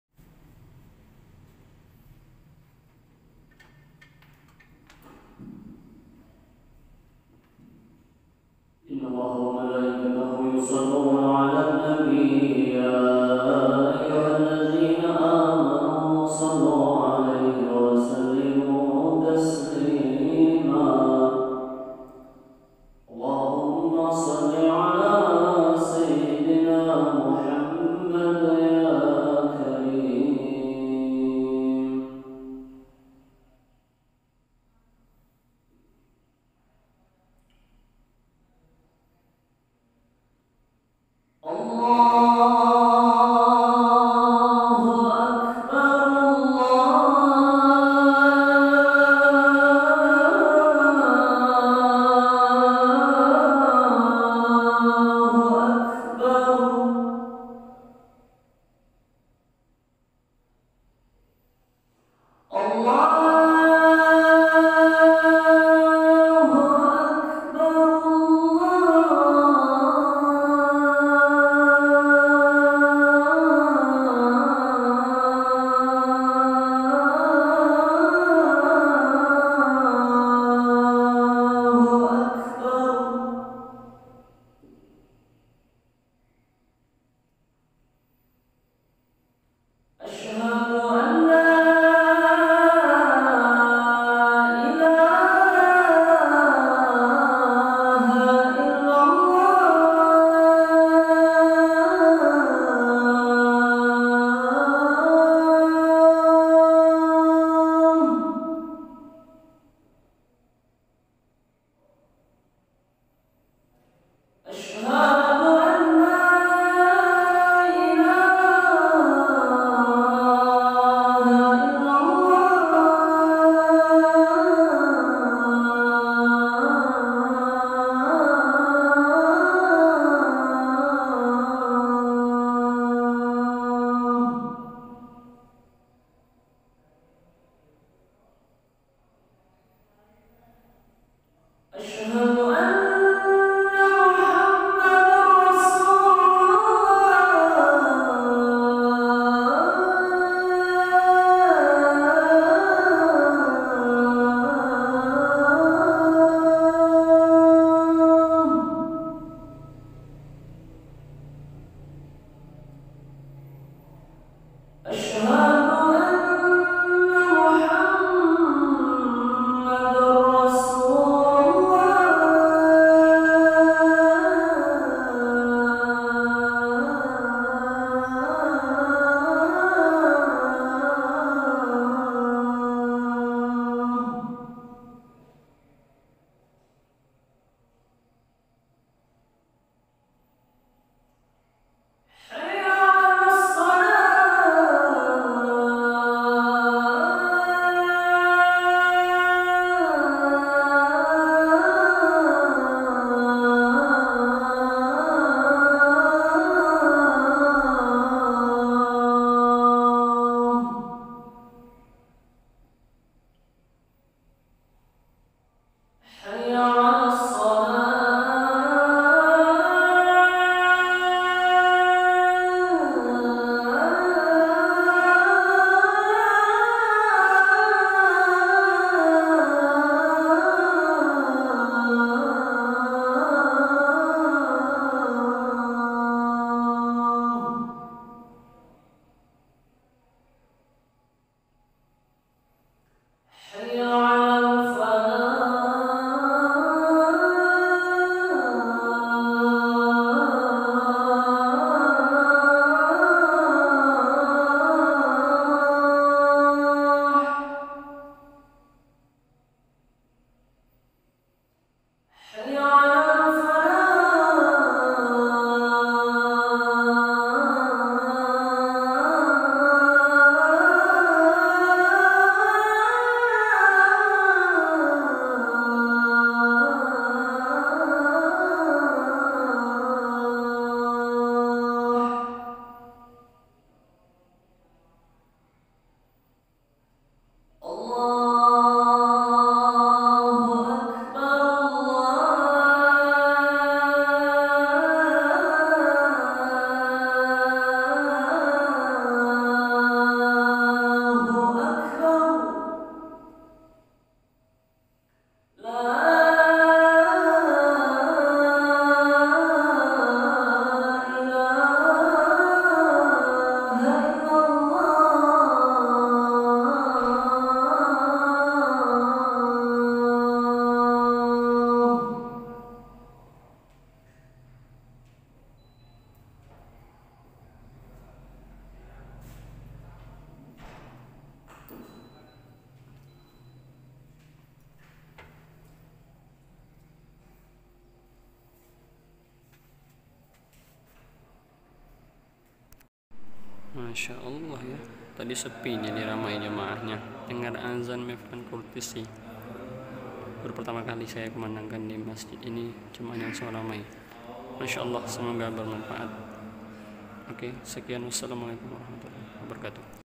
Heboh Adzan Kurdi Buat Penasaran Belum Pernah Didengar Disini Jamaah Sepi Langsung Rame
Adzan Kurdi versi Mevlan Kurtishi buat penasaran Jamaah awalnya Sepi langsung pada Semangat Berdatangan